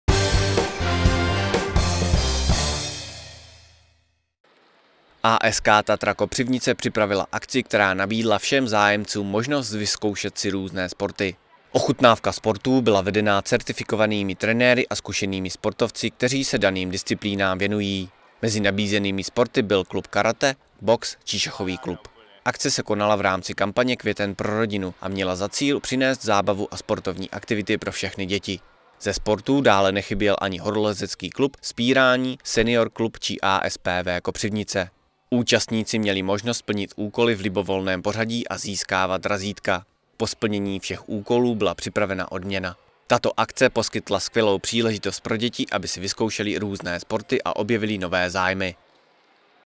02 Zprávy 06 Žijte sportem
Akce „Žijte s námi sportem“ v rámci kampaně Květen pro rodinu.